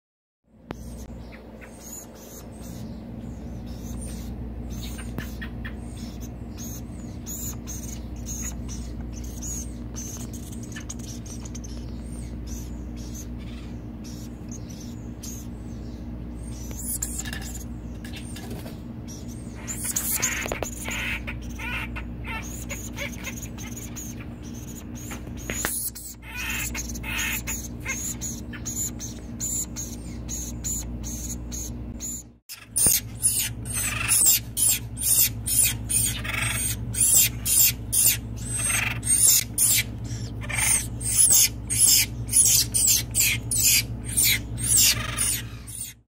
Acorn Woodpeckers
A peek into the life of Acorn woodpeckers in rehab at The Bird Rescue Center.

The Bird Rescue Center
3430 Chanate Rd, Santa Rosa, CA, 95404